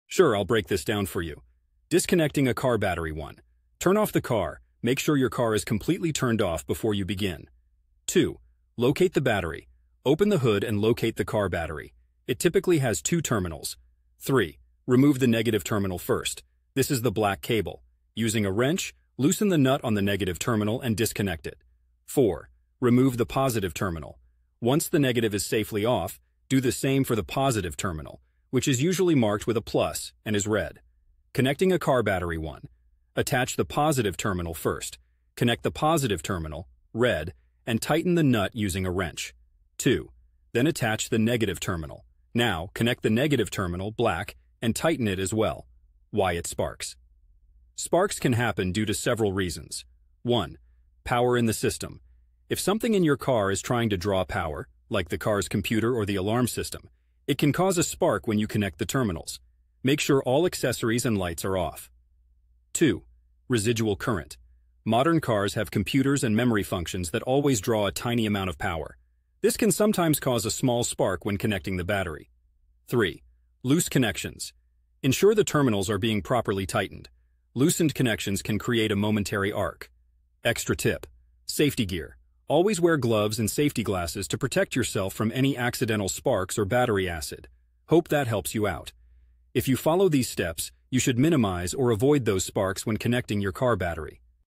Why Did My Car Battery Spark When Connecting Negative Terminal?
I answered this question on Quora My video answer to the question: How Does One Connect And Disconnect A Car Battery I Am Asking Because I Connected The Positive First Then The Negative But While Trying To Connect The Negative It Would Spark Any Ideas Why That Would Happen?